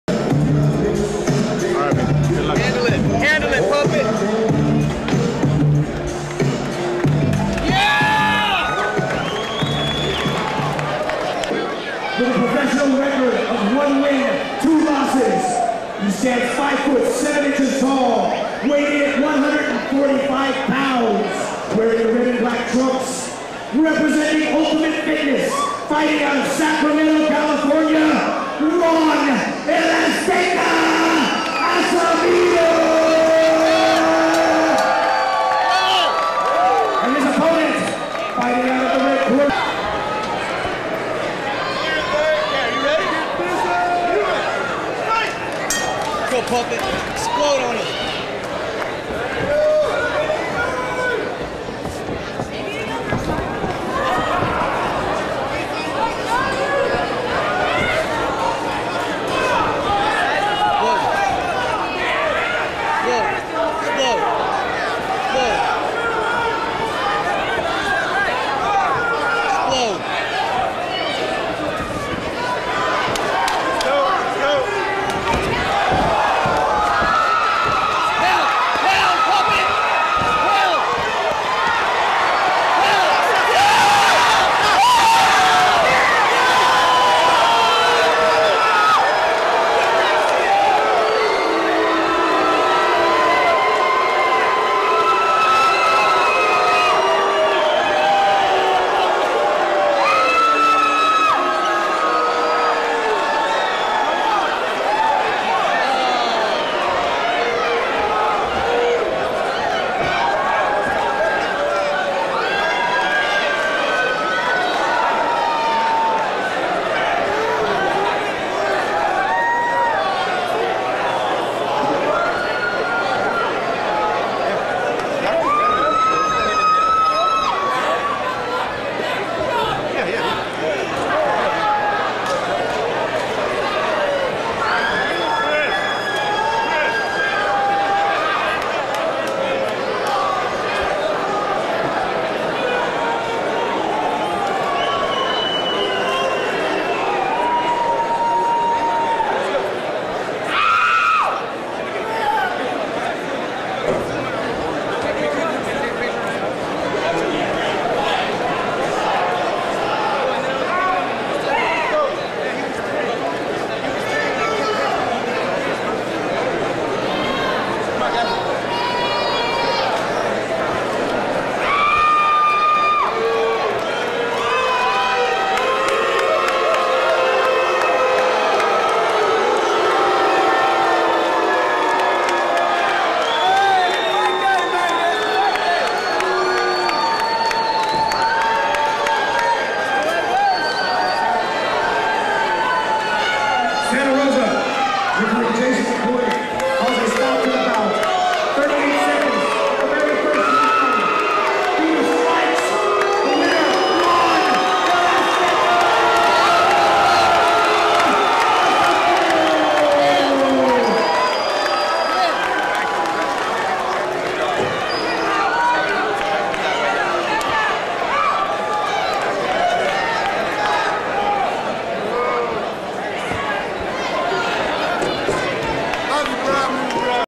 Juan "El Azteca" Acevedo vs Daniel Mitchell CAGE COMBAT
Juan "El Azteca" Acevedo with a first round win in :38 seconds over a respected fighter in Daniel Mitchell.